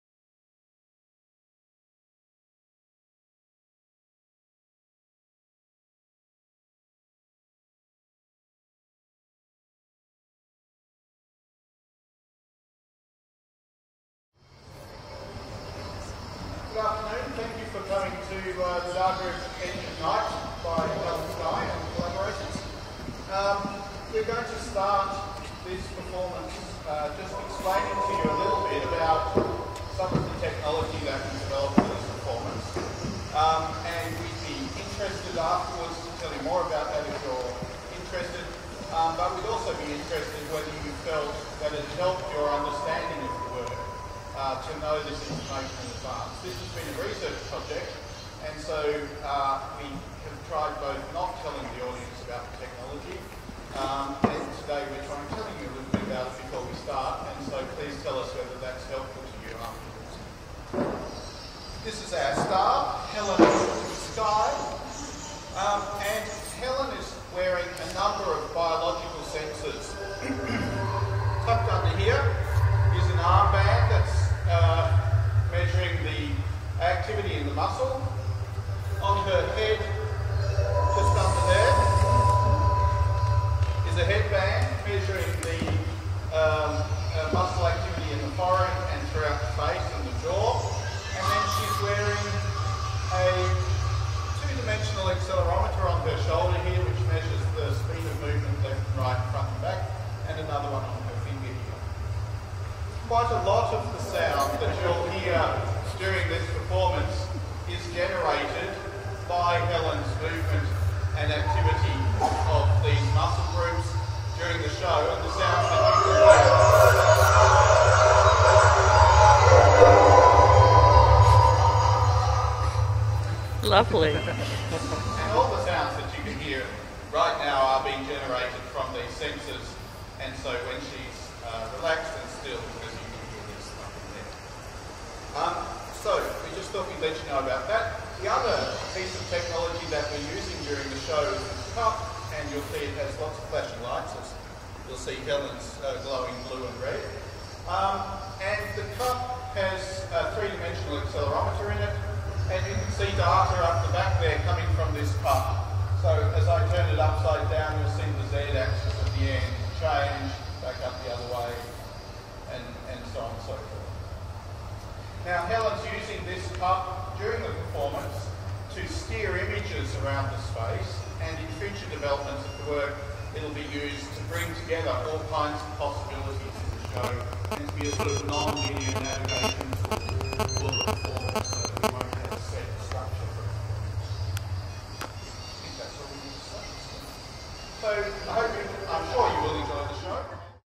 Darker Edge of Night - introduction to interactive bio sesning technology used in the performance
Darker Edge of Night (Performance) Melbourne Meat Market  Australia 08
 interactive performance - using  real time  bio sensing, to audio visual projection melbourne australia
Hellen Sky, T.J.Bateson, Margaret Cameron, Paul Bourke, David Franzke, Susie Fraser, Rebecca Hilton, Leigh Warren, Garth Paine, Michael Pierce, Dianne Reid, Tetsua Tabasha, Brandon Hur, Sarah Rubidge, BlueBottle.

Supported by Australia Council for the Arts, Interarts ArtsLab, CultureLab ArtHouse City of Melbourne 2008, Research Centres VIPRe (Virtual Interactive Performance Research Environment) UWS, WASP (Western Australia Supercomputing Program) UWA, Deakin Interns.

Video Documentation Dianne Reid